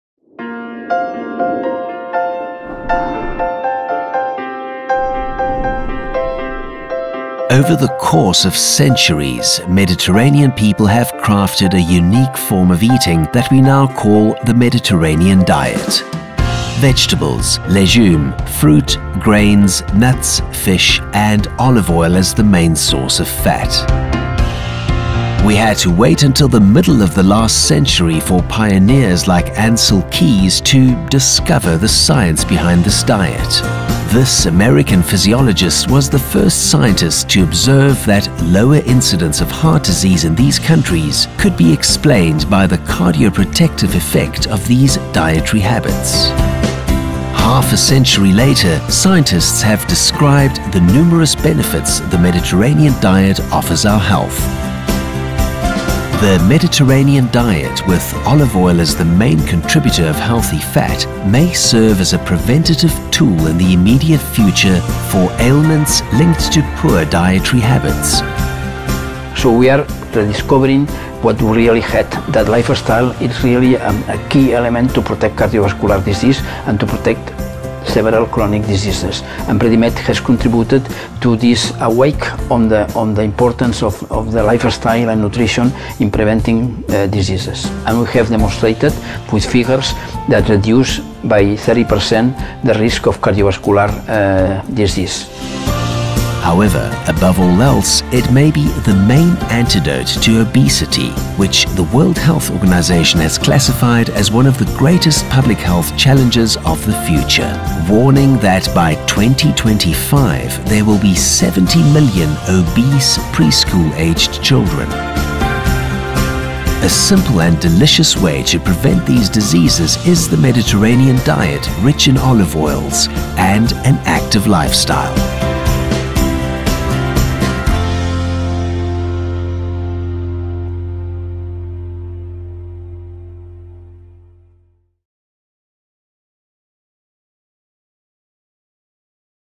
エキストラバージンオリーブオイルの効能を発見
PREDIMEDの研究は、エキストラバージンオリーブオイルに富んだ地中海食が体の機能に様々な利益をもたらすことを科学的に実証しました。詳しく知りたいですか？

The Mediterranean Diet, rich in extra virgin olive oil, is one of the healthiest dietary models available, especially given its ability to reduce the risk of a cardiovascular event by up to 30.

Follow us and discover all the secrets of Olive Oils from Spain!